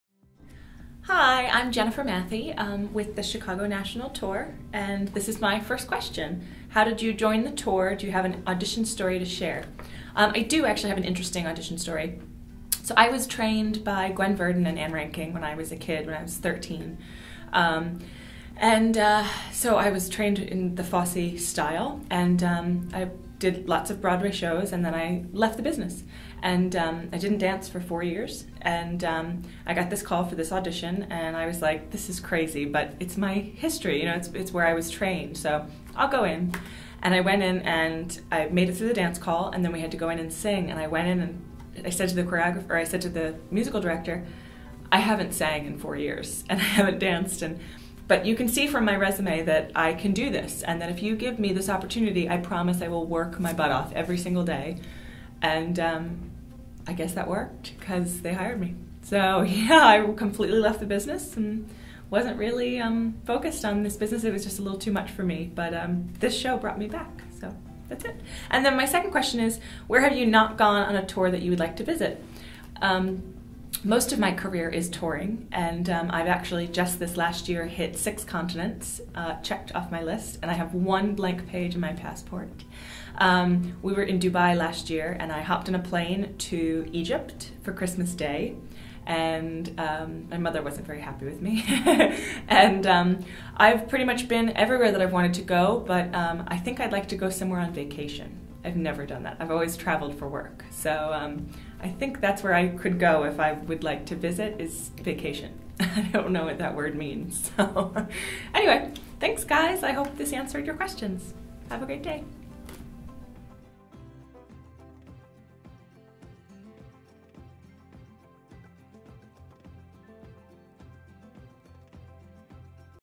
Broadway Backstage: Jennifer Mathie of "Chicago"
Jennifer Mathie, a swing with the touring production of "Chicago," shares her audition story, and where she would like to go on tour (she has one blank page on her passport!).